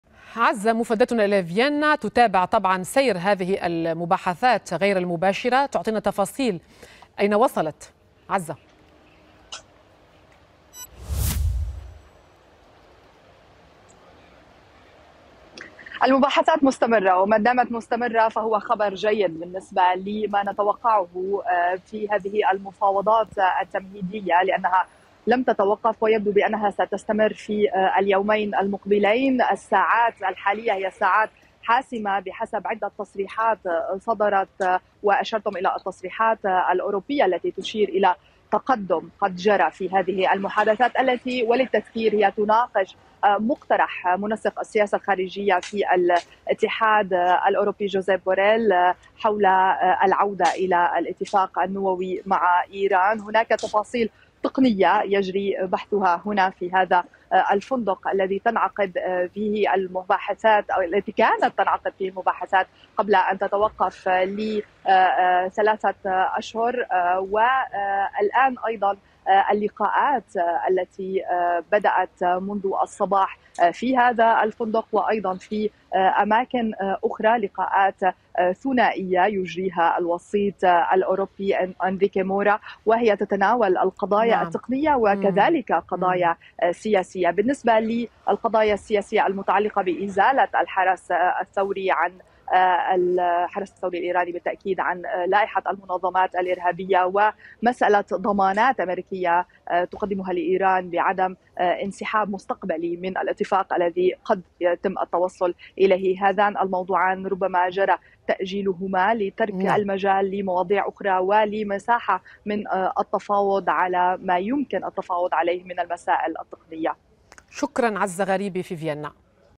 مباحثات إيرانية أميركية غير مباشرة للعودة للاتفاق النووي
أعلنت وسائل إعلام إيرانية أن الوفد الإيراني إلى فيينا قد عقد محادثات غير مباشرة مع الجانب الأميركي تناولت سبل العودة للاتفاق النووي المبرم عام الفين وخمسة عشر.
هذا وكشف مسؤول كبير في الاتحاد الأوروبي عن إحراز تقدم بشأن بعض العقبات المتبقية في المفاوضات.
من فيينا موفدتُنا عزة غريبة